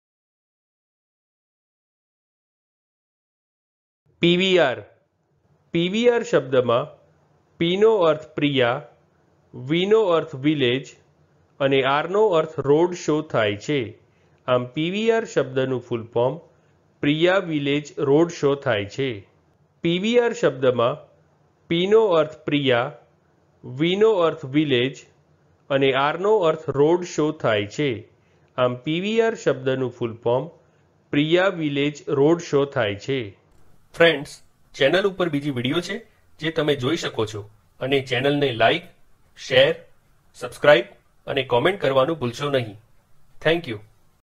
PVR Full Form in Gujarati | PVR નું ફૂલ ફૉર્મ શું છે
આ વિડિયો માં તમે PVR નો Gujarati માં full form સમજશો અને એની સાથે PVR નું pronunciation પણ શિખશો. એટ્લે કે તમે PVR ના full form ની સાથે સાથે એ પણ શિખશો કે PVR નો અર્થ શું થાય છે અને PVR ને બોલવાની સાચી રીત કઈ છે.

 તો બસ એક મિનટ માં શીખો PVR શબ્દ ને.